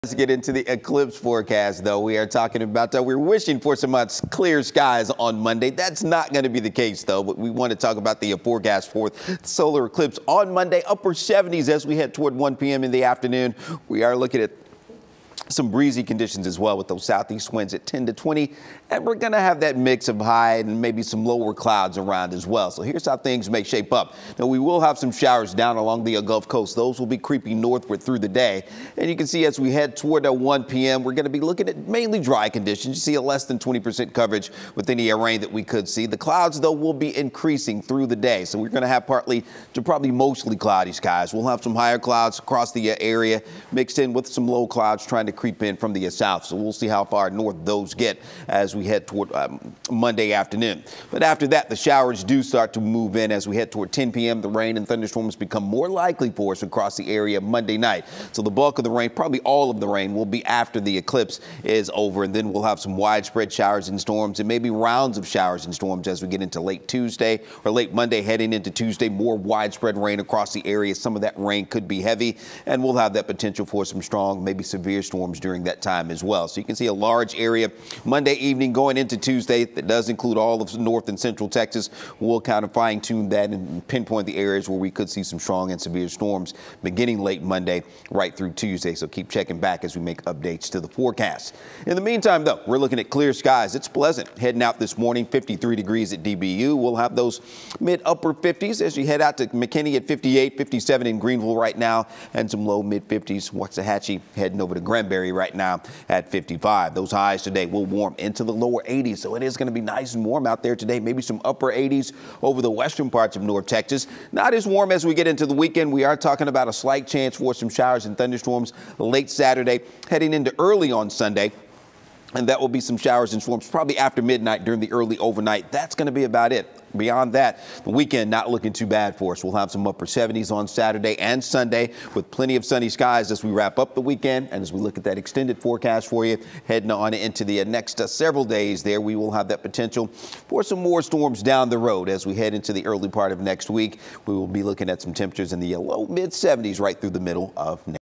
DFW total solar eclipse forecast: Latest weather updates
Greg Fields has a look at the latest eclipse forecast for North Texas.